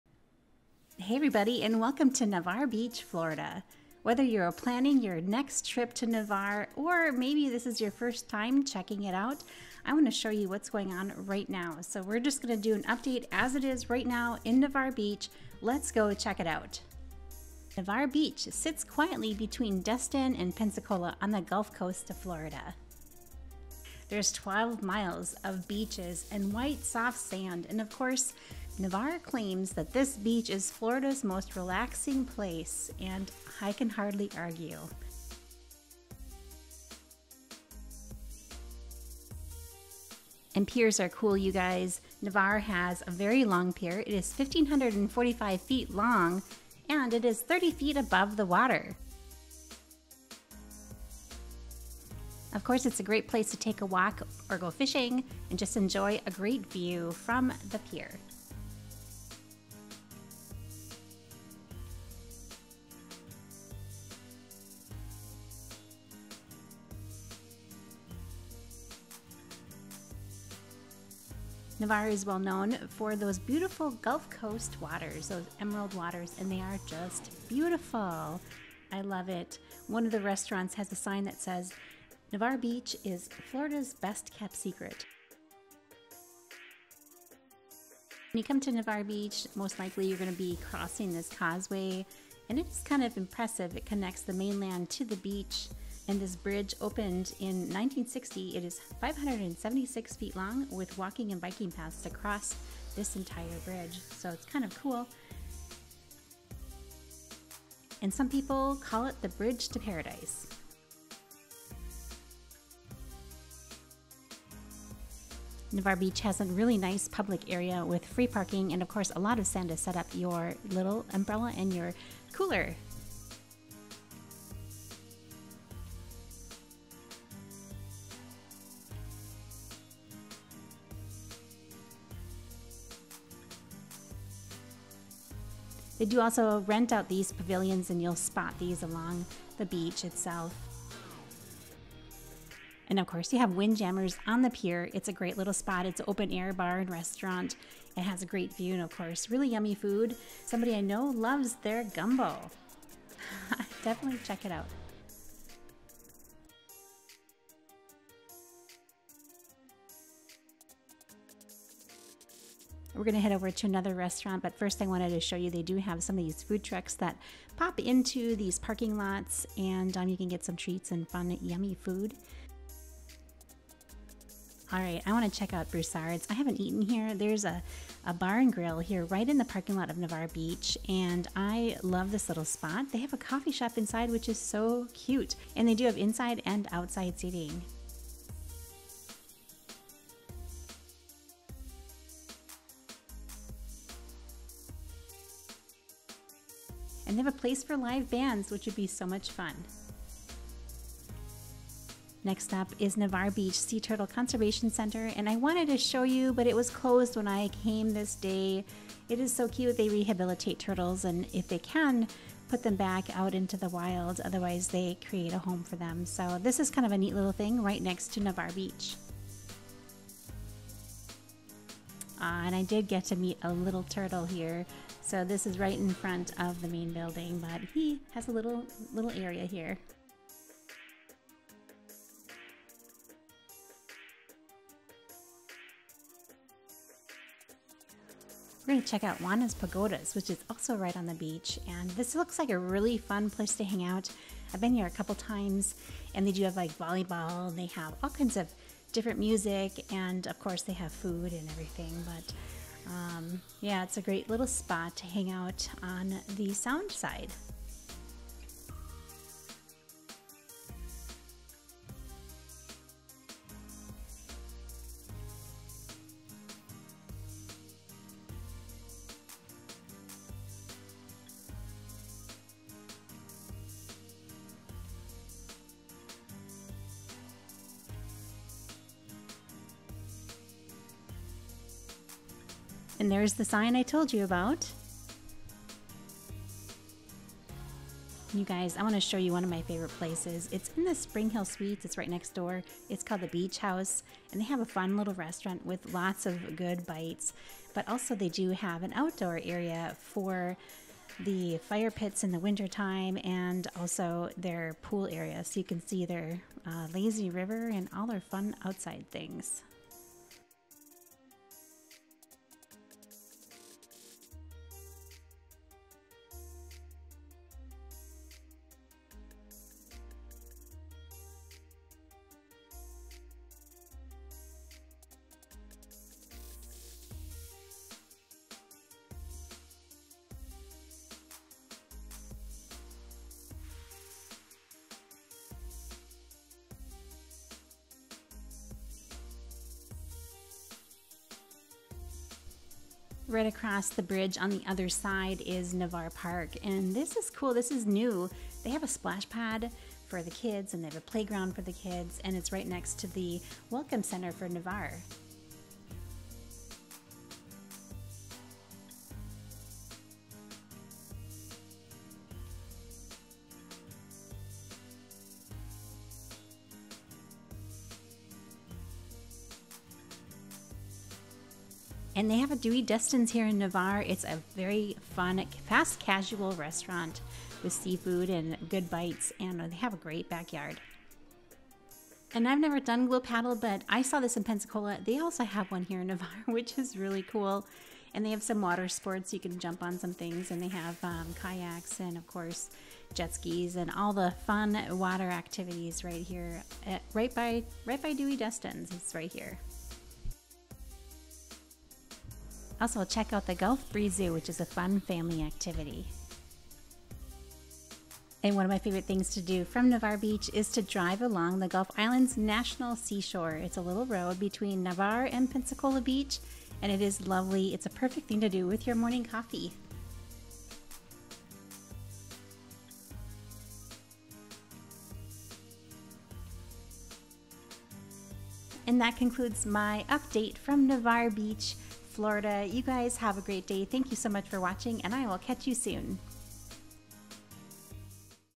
Navarre Beach, Florida 2023
Hi Everyone! I love the Gulf Coast, so many beautiful beaches to visit! Come along with me to check out Navarre Beach and some of my favorites in Navarre! Maybe you're in planning mode or excited on your next visit. It is Florida's Best Kept Secret!

@GulfCoastGirlyoutube on Facebook

FAA Part 107 Licensed Drone Pilot